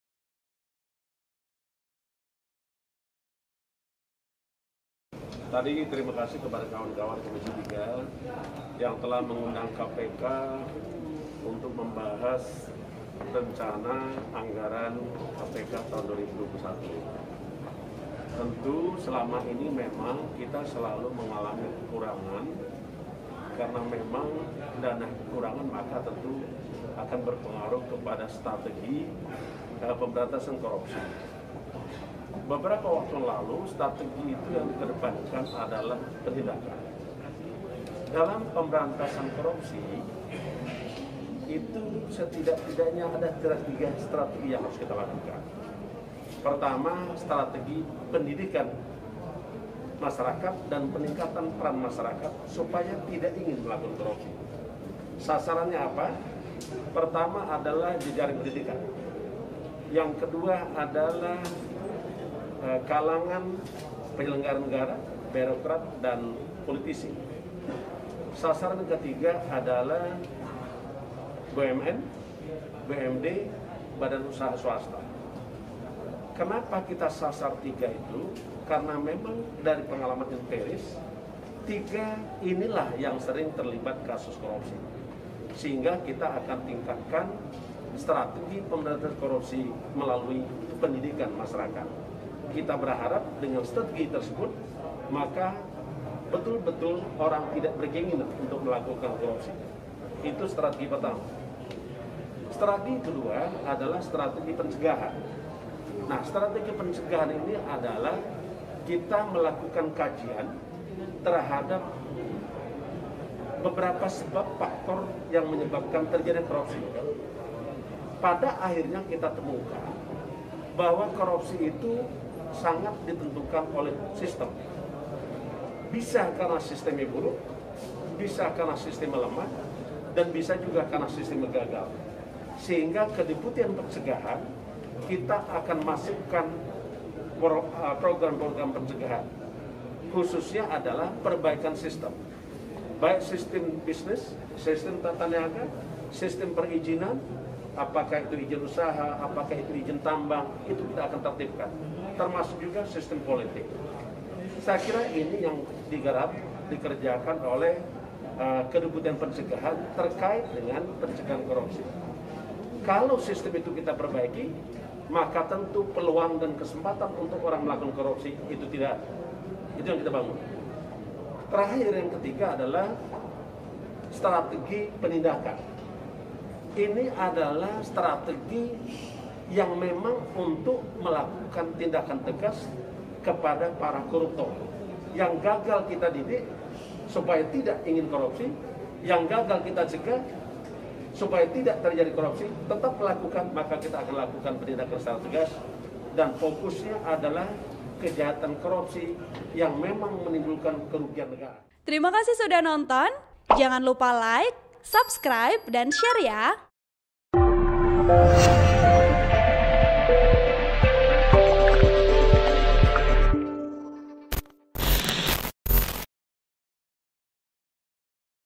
Ketua KPK: Korupsi Sangat Ditentukan oleh Sistem yang Buruk, Lemah dan Gagal
Laporan Wartawan Tribunnews.com, Chaerul Umam

TRIBUNNEWS.COM, JAKARTA - Ketua KPK Firli Bahuri menjelaskan tiga strategi dalam upaya pemberantasan korupsi.

Satu diantaranya yaitu strategi pencegahan. Firli mengatakan strategi ini melakukan kajian terhadap beberapa sebab faktor yang menyebabkan terjadinya korupsi.

"Pada akhirnya kita temukan bahwa korupsi itu sangat ditentukan oleh sistem, bisa karena sistemnya buruk bisa karena sistemnya lemah dan bisa juga karena sistemnya gagal," kata Firli usai rapat dengan Komisi III DPR, di Kompleks Parlemen, Senayan, Jakarta, Kamis (25/6/2020).

"Sehingga kedeputian pencegahan kita akan masukkan program-program pencegahan khususnya adalah perbaikan sistem baik sistem bisnis, sistem tata niaga, sistem perizinan izin usaha, izin tambang. Kita akan tertibkan termasuk juga sistem politik," imbuhnya.

Strategi lainnya adalah melalui pendidikan masyarakat dan peningkatan peran masyarakat.

Firli mengatakan strategi itu menyasar jejaring pendidikan, kalangan penyelenggaran negara birokrat dan politisi dan kalangan BUMN, BUMD dan badan usaha swasta.

"Kenapa kita sadar ketiga itu? karena memang dari pengalaman empiris tiga inilah yang sering terlibat kasus korupsi, sehingga kita akan tingkatkan strategi pemberantasan korupsi melalui pendidikan masyarakat. Kita berharap dengan strategi tersebut maka betul-betul orang tidak berkeinginan untuk melakukan korupsi," kata Firli.

Strategi terakhir adalah strategi penindakan. Firli mengatakan strategi ini adalah untuk melakukan tindakan tegas terhadap para koruptor.

"Kita akan melakukan penindakan secara tegas dan fokusnya adalah kejahatan korupsi yang memang merugikan negara. Pada prinsipnya pemberantasan korupsi itu adlah mencegah dan mengembalikan kerugian negara," pungkas Firli.